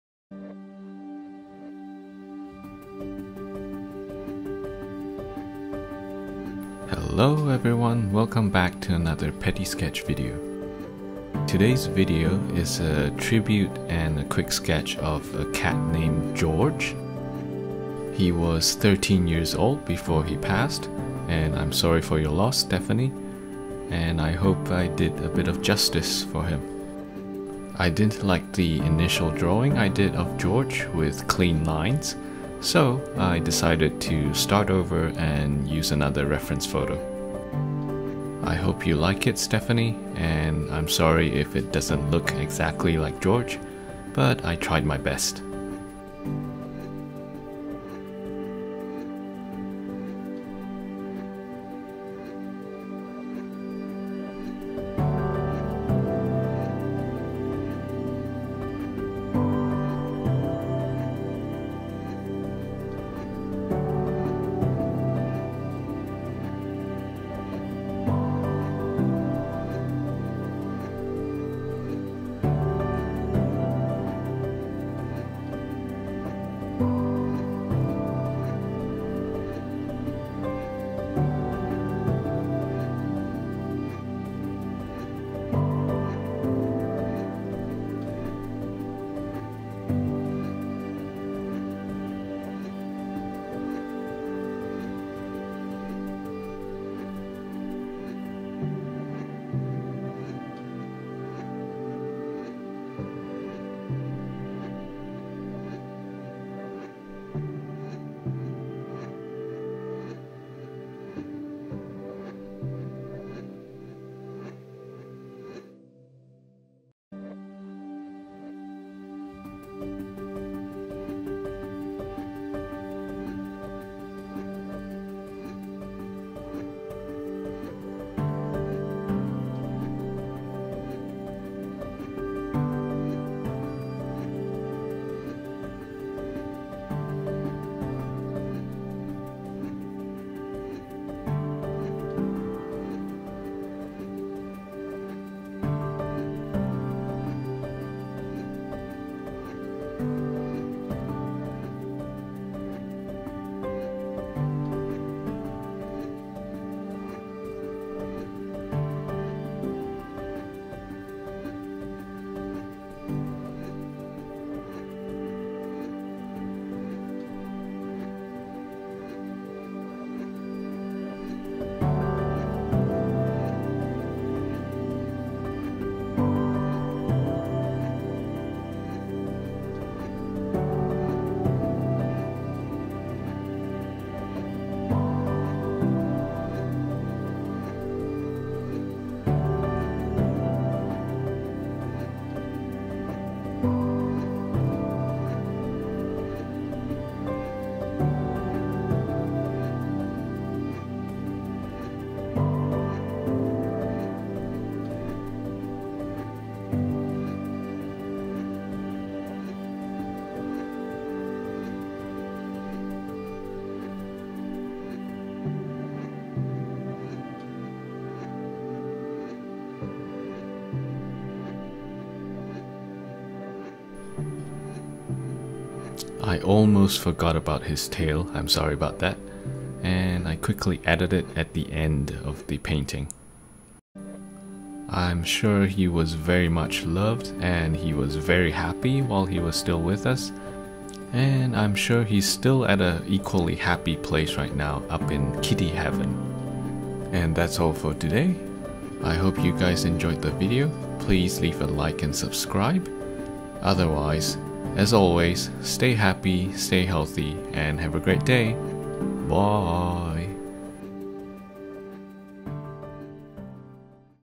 Petty Sketch #21 | George tribute quick sketch
Hi everyone, I did a quick sketch of George as a tribute today. He was 13 years old when he passed recently. My condolences to Stephanie for her loss. I tried my best to capture him in a sketch, hope I did him justice! Hope you guys enjoy the video!